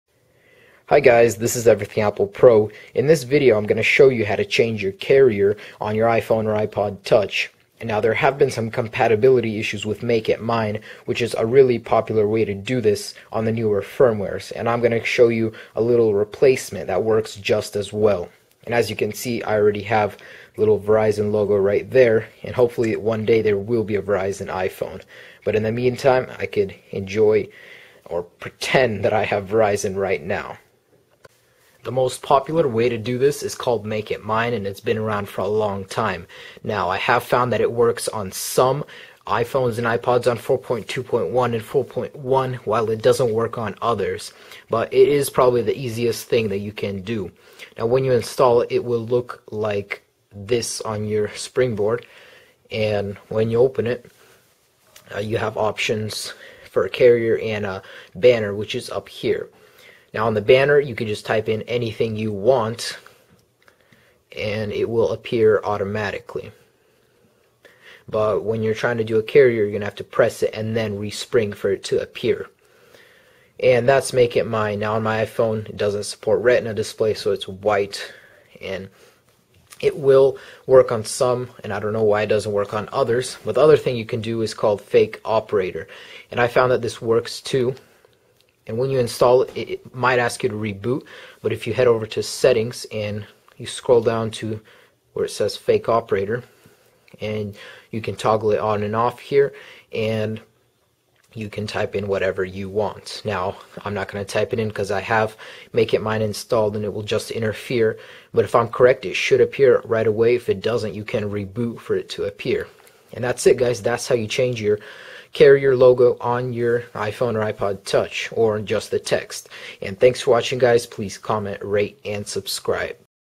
How To Change Carrier On iPhone & iPod Touch
Want To Change Your Carrier To Verizon Or Anything Else? Watch This Video To Learn How!

Makeitmine Or Fake Operator Or iName

IF NONE Of These Are Working Try iName Which Works For 4.2.1